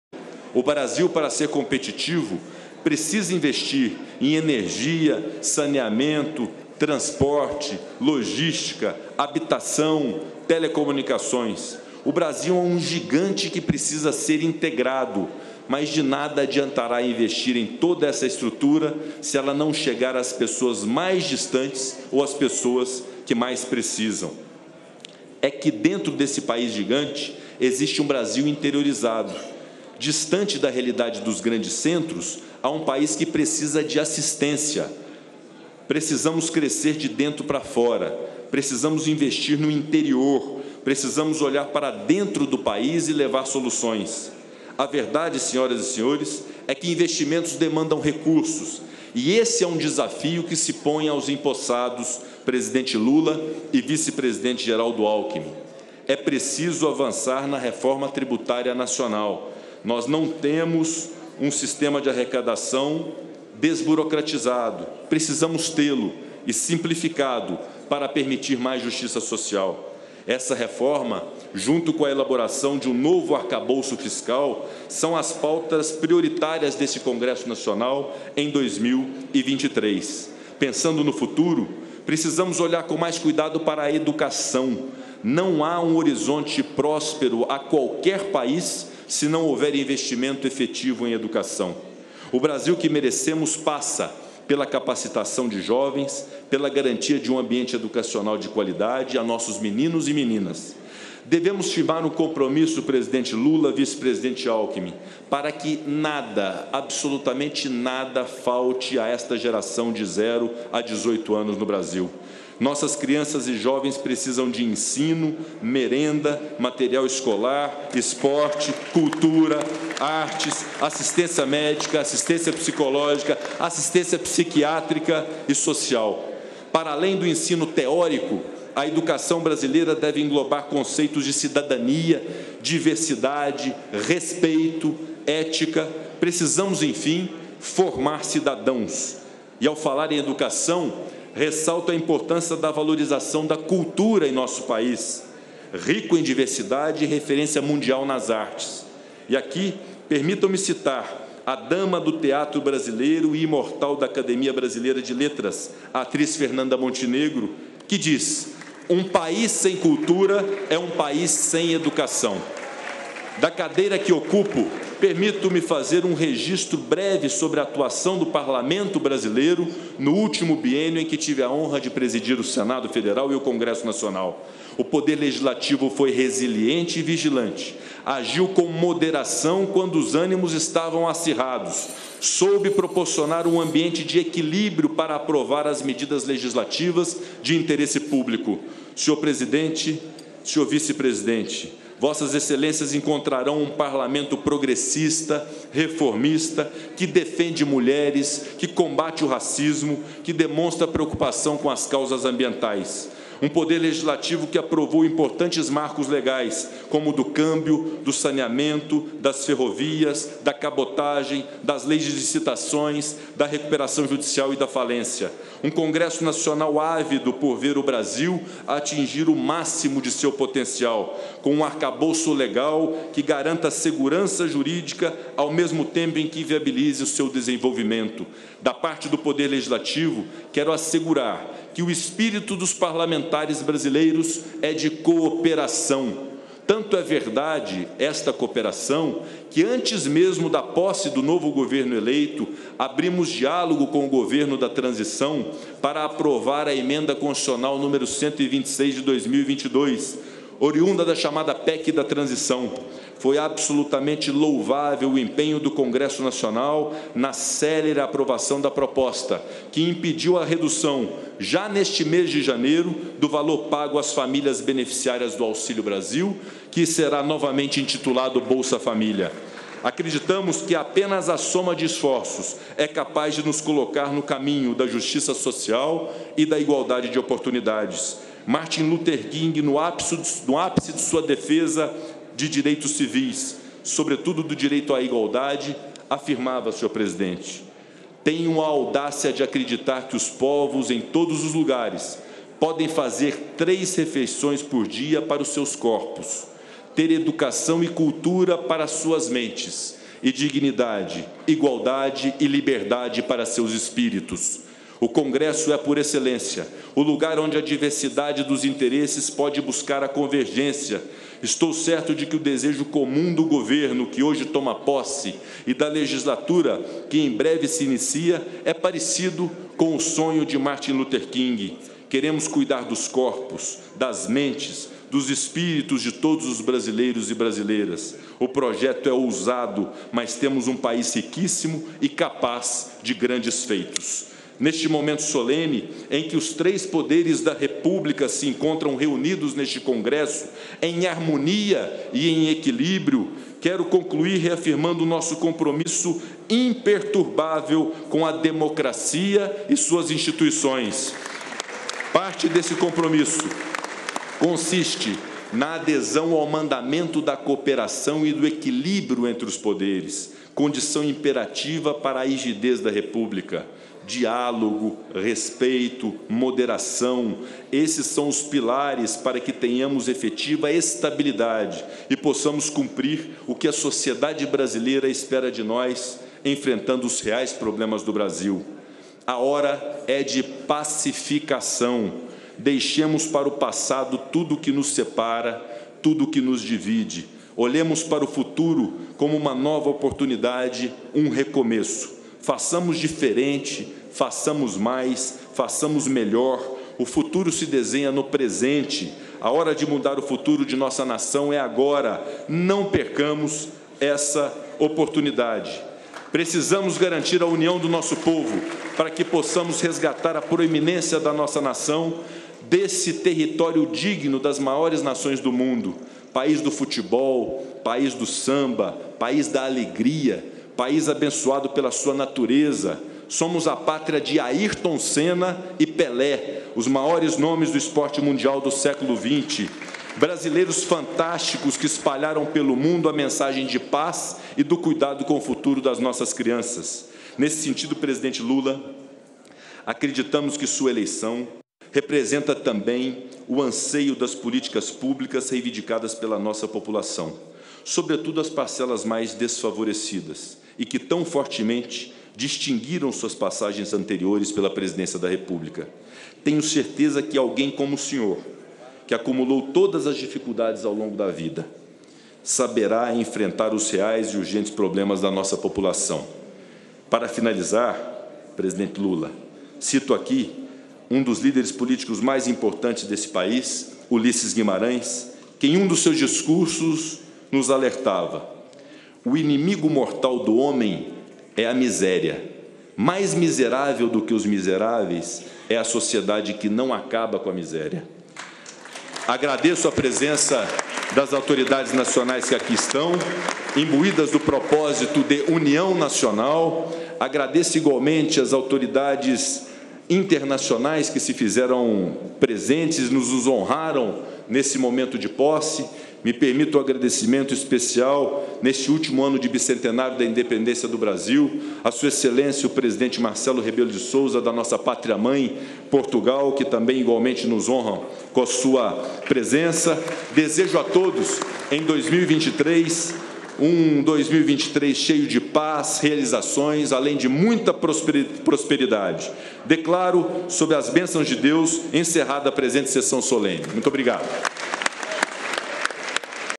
Rodrigo Pacheco encerra pronunciamento na posse presidencial de Luiz Inácio Lula da Silva
"O Brasil é um gigante que precisa ser integrado, mas nada adianta se o investimento não chegar nas pessoas necessitadas".